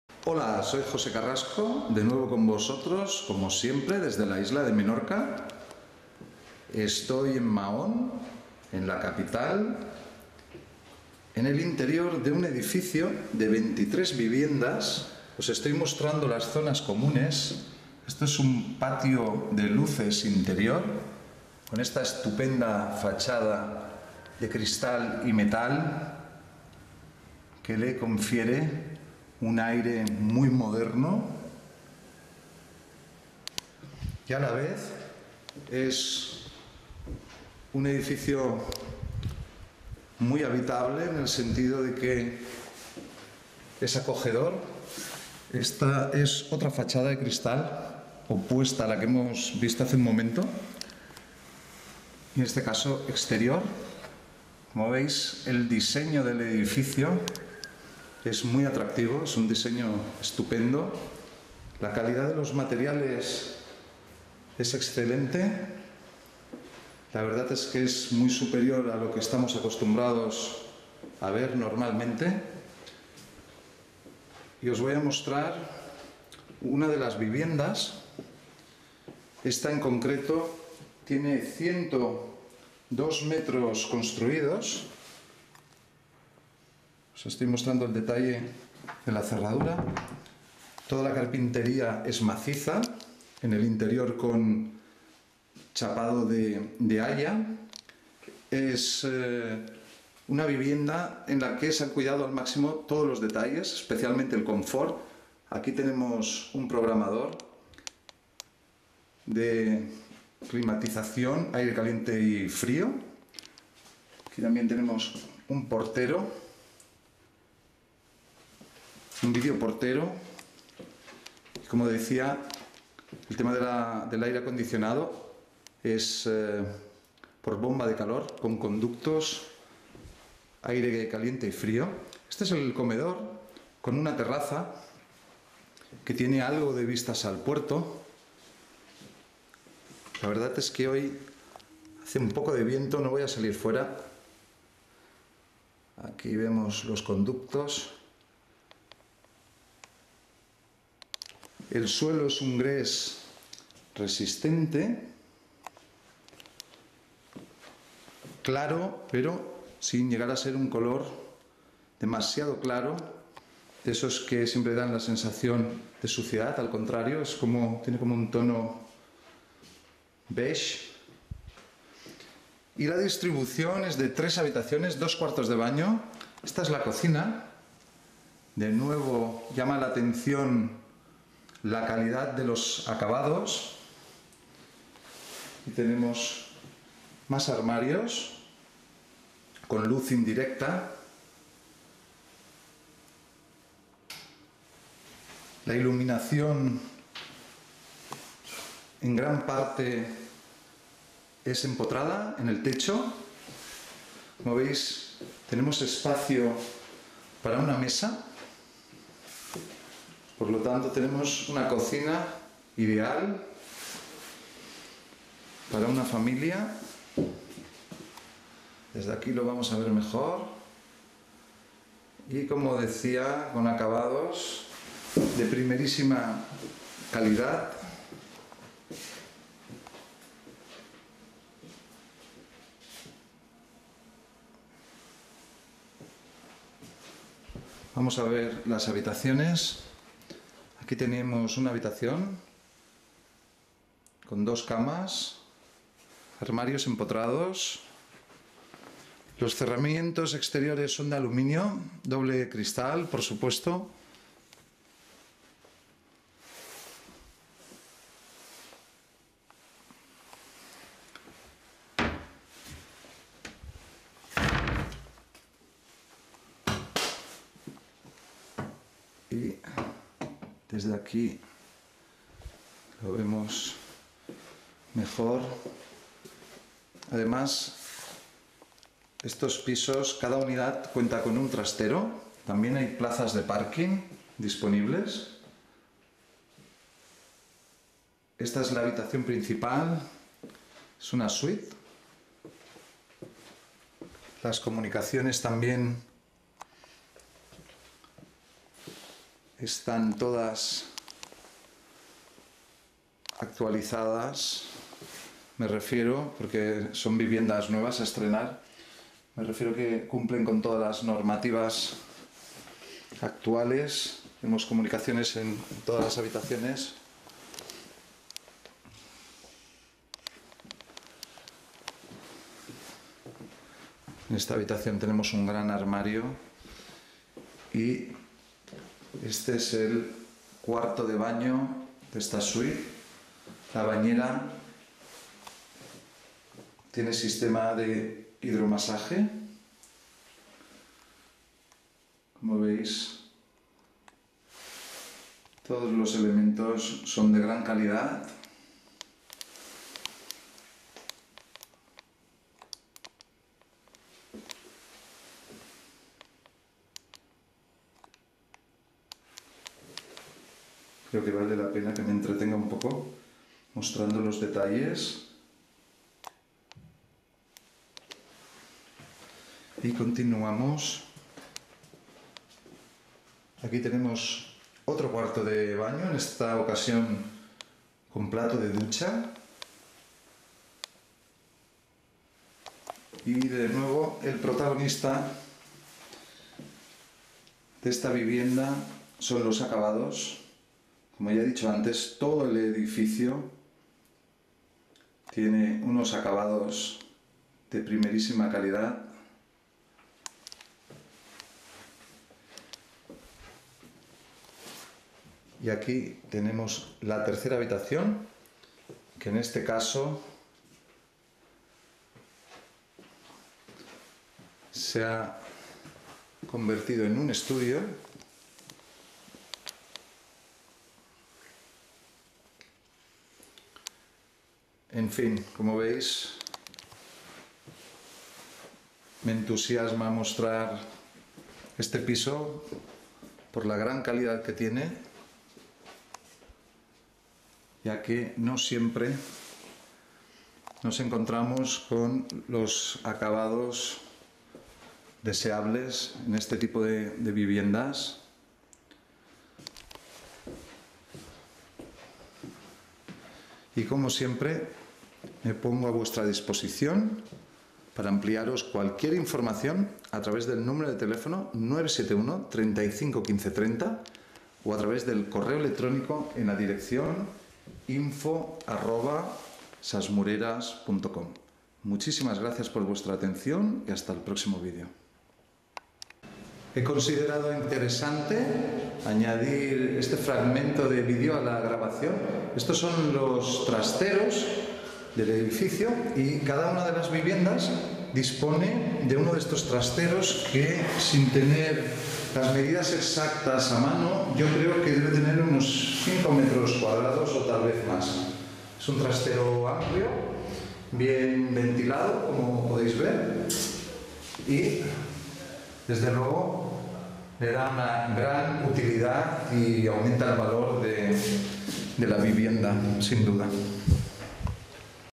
Estupendo piso en Mahón-Menorca con muy buenos acabados
Estupendo piso de 3 habitaciones, trastero y posibilidad de parking con acabados de lata calidad en un edificio de diseño cerca del puerto de Mahón.

Precio y fotografáis en el enlace debajo

Great 3 bedroom apartment with with utility room, high quality finishes on stylish building near Mahon port.